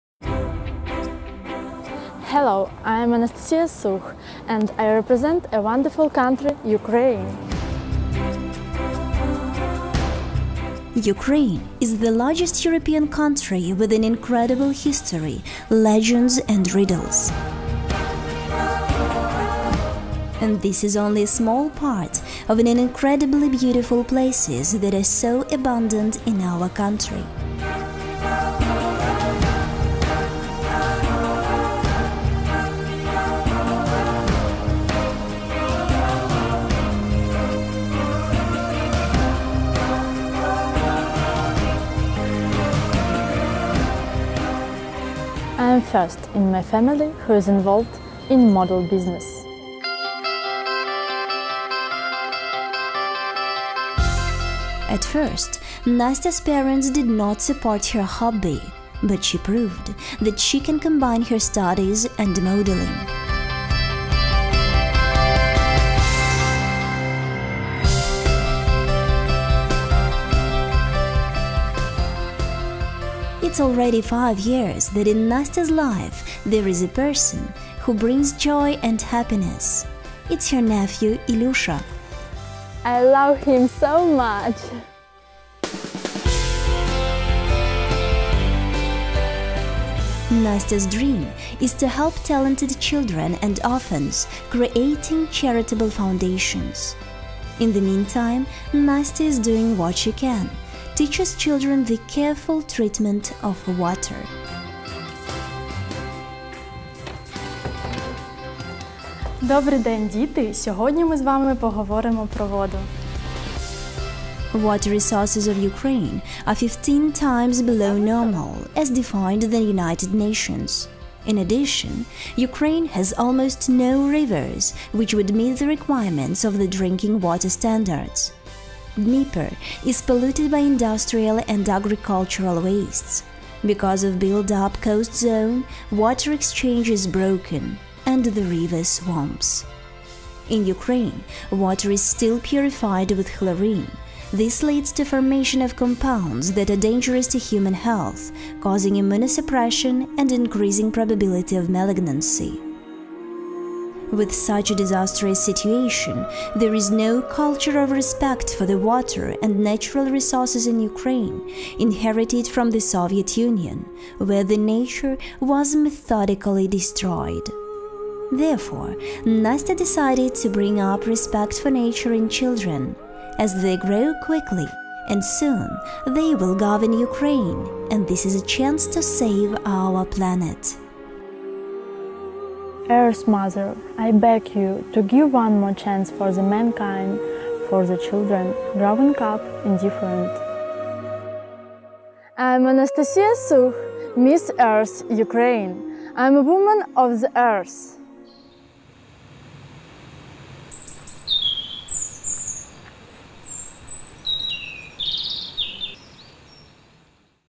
КТО "АКАДЕМИЯ ЗВЕЗД" DM "DEFILE MODELS`CITY"
СЪЕМКИ ВИДЕОРОЛИКА ДЛЯ МЕЖДУНАРОДНОГО КОНКУРСА КРАСОТЫ "МИСС ЗЕМЛЯ, 2013" - АНАСТАСИЯ СУХ